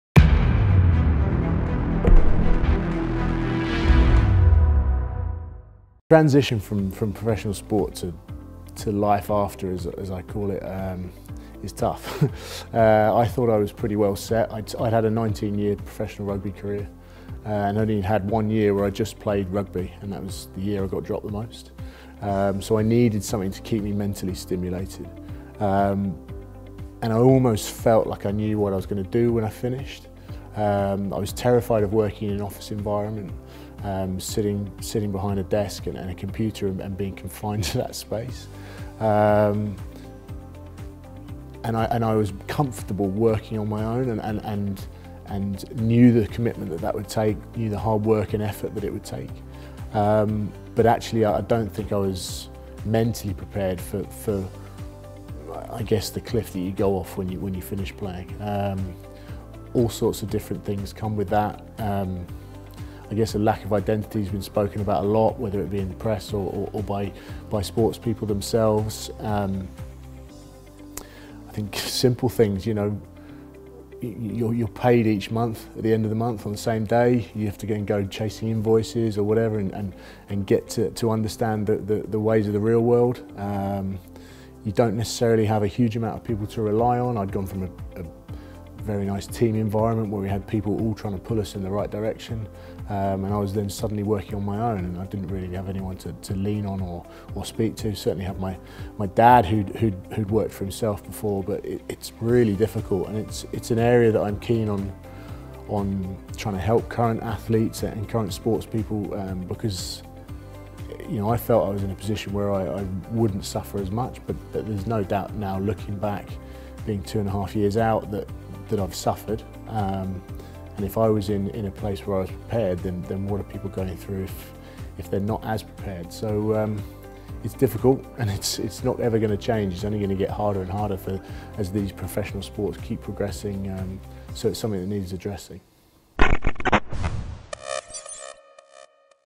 Tom May: Transitioning to life after sport
Former England Rugby player, now partner at Club Sportif